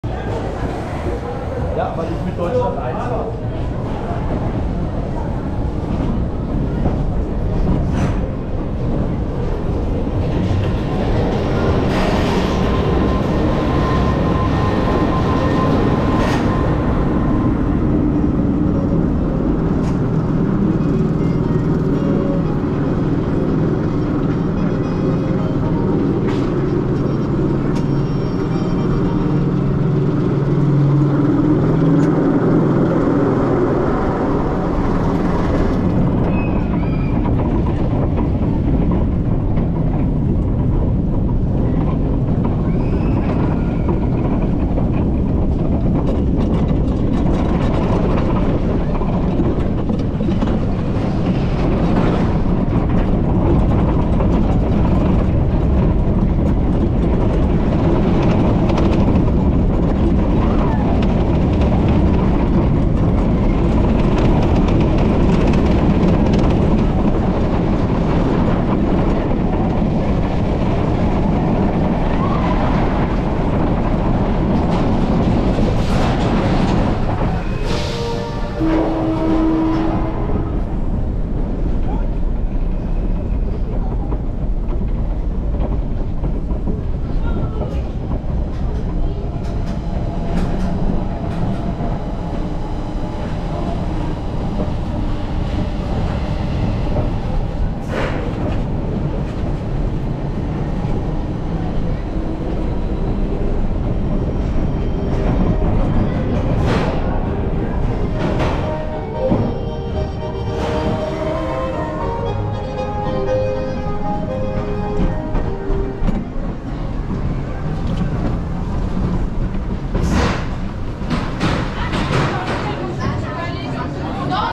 Schweizer Bobbahn POV (Back Row, 4K 60FPS, 2024), Europa Park Mack Bobsled Coaster | Non-Copyright
Schweizer Bobbahn (Swiss Bob Run) at Europa Park was the prototype Mack bobsled.  Located in the Switzerland section of the park, this coaster doesn't have the longest layout, but it's integrated beautifully into the area.